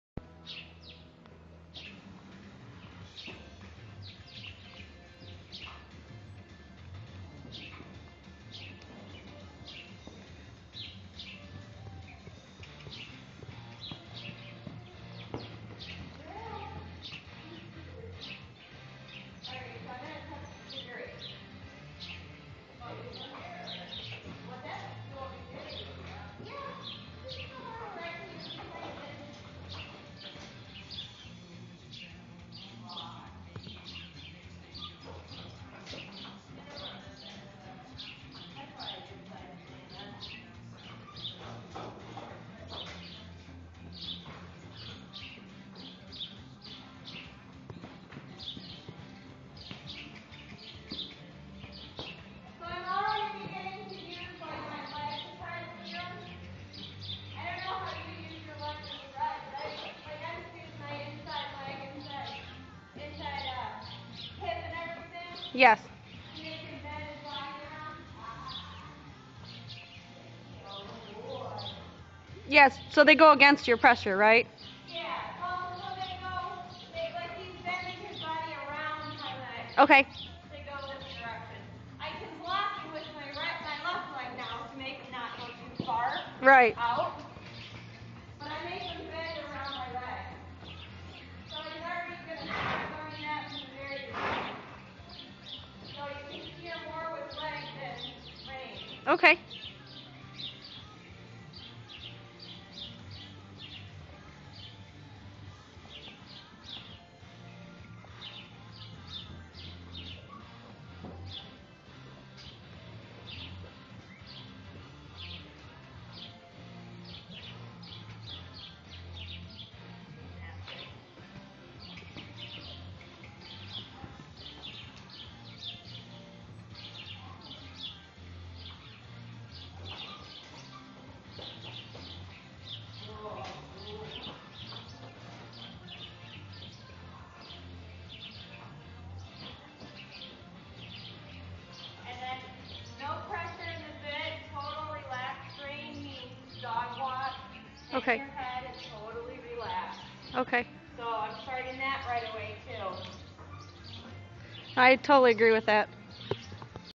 Stallion Homozygous Spotted TWH & Spotted Saddle Horse
Stallion - 3 Year old Homozygous Tobiano Spotted TWH & Spotted Saddle Horse.
5th day under saddle. 4.5.11